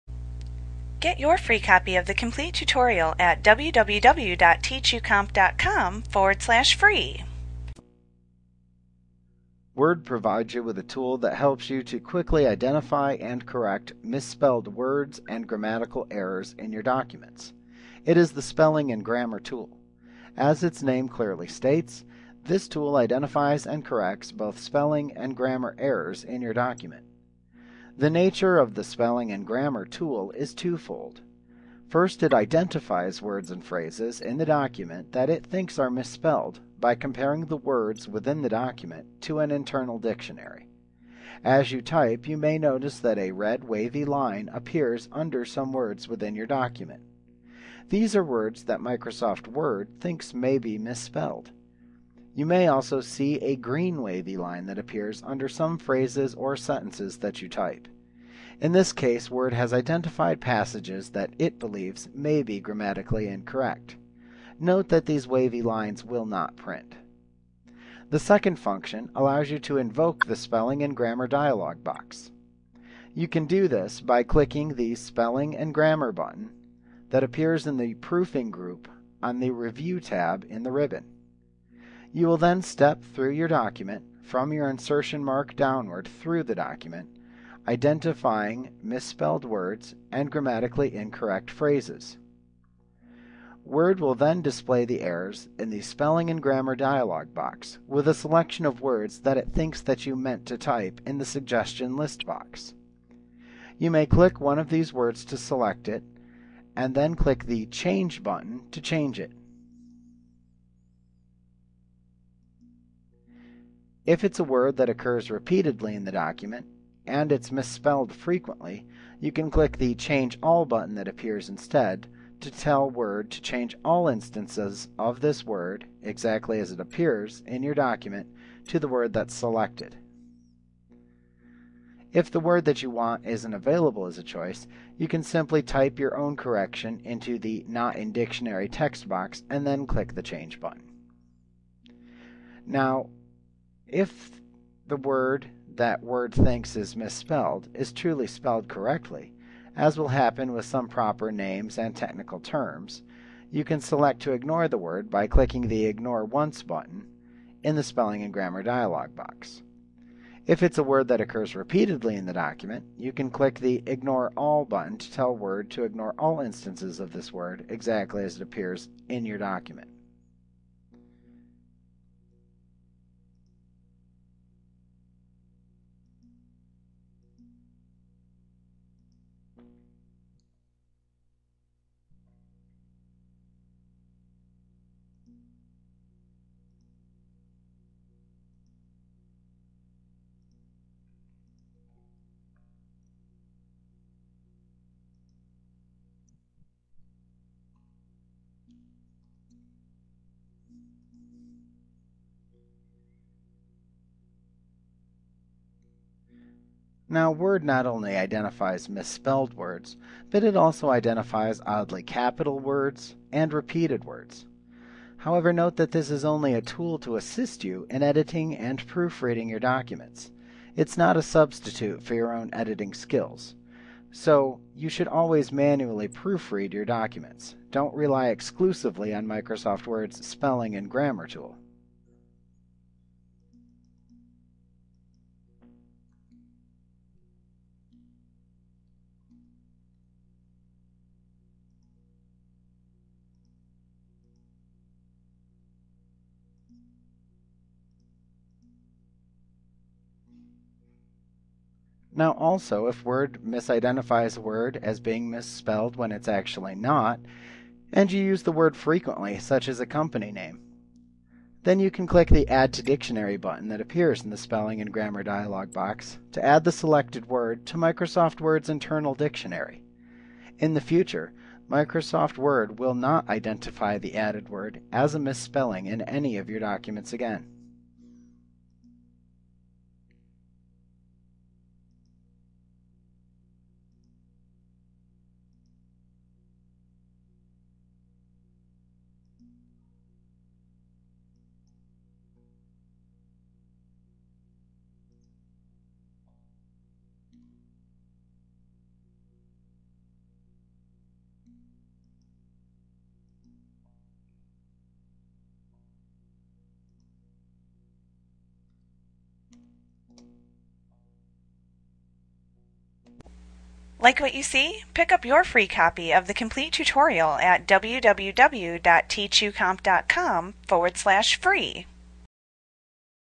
Word 2010 Tutorial The Spelling and Grammar Tool Microsoft Training Lesson 4.1
Learn about the spelling and grammar tool in Microsoft Word - the most comprehensive Word tutorial available. Visit us today!